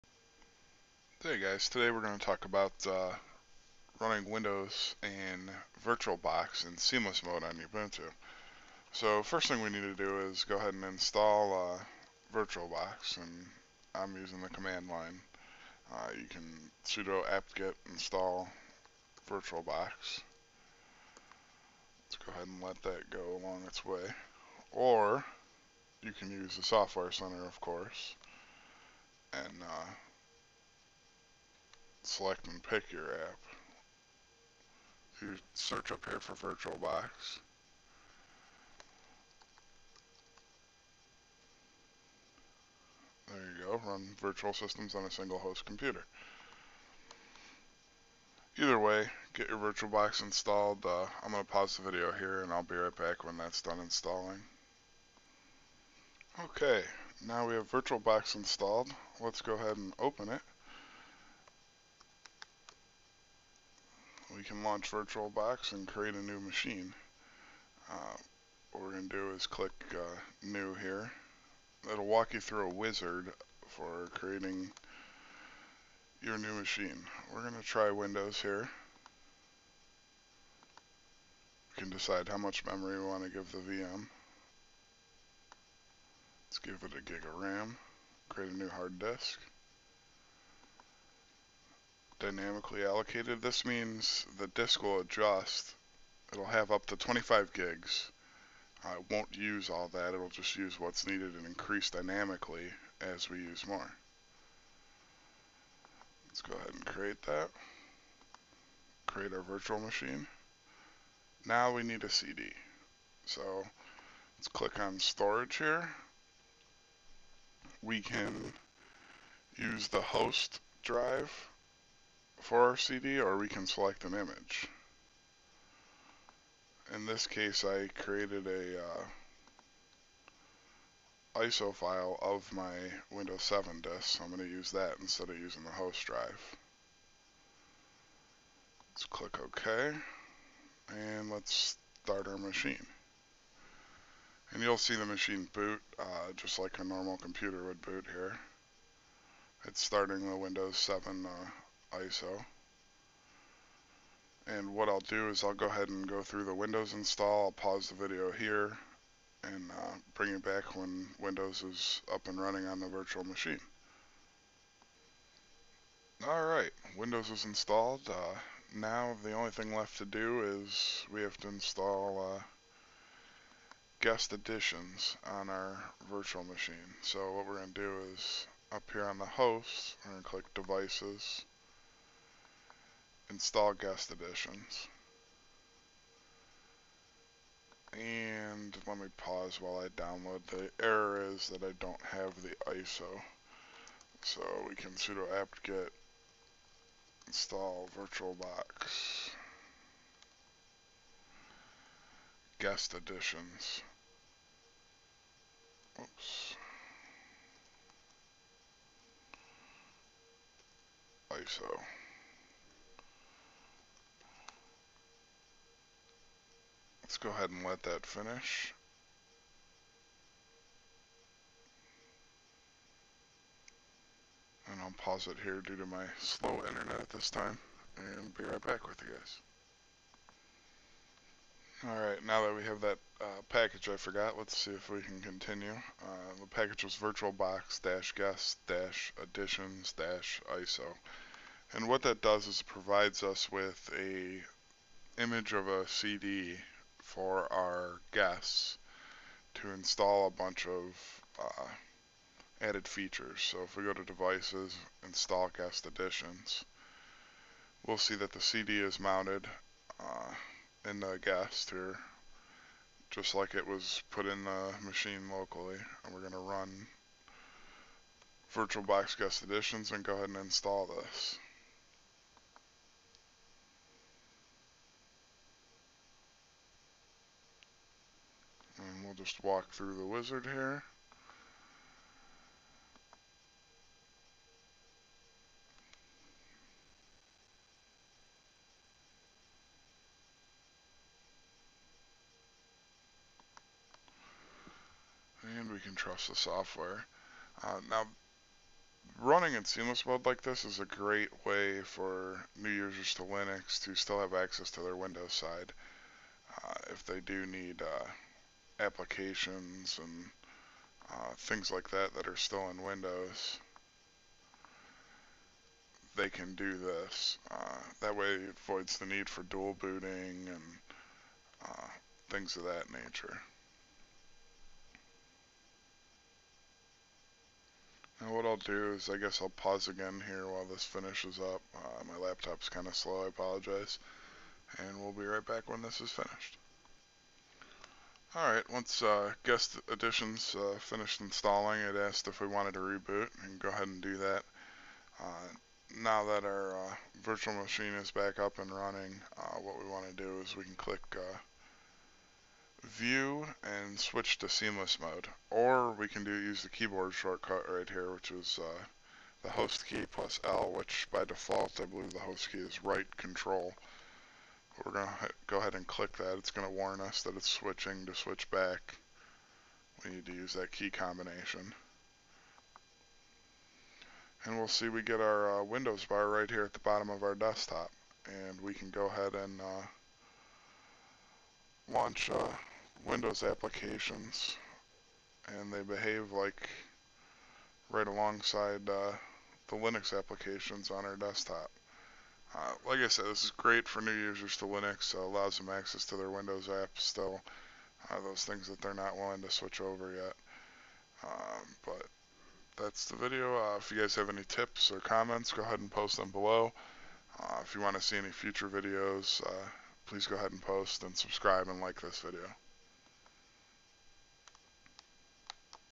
Howto: Ubuntu 12.04 running Windows 7 in Virtualbox seamless mode
A short video showing howto run a Windows virtual machine with virtualbox in seamless mode inside of Ubuntu.

Commands used:
sudo apt-get install virtualbox
sudo apt-get install virtualbox-guest-additions-iso
create a windows virtual machine and install windows
Install guest additions in the guest Right Ctrl+D
Switch to seamless mode Right Ctrl+L

Drop down terminal used is guake you can find it in the Ubuntu repos.

You cannot run games this way performance is horrible due to virtual video card. This is currently the only work around for watching Netflix in Linux is with a virtual machine.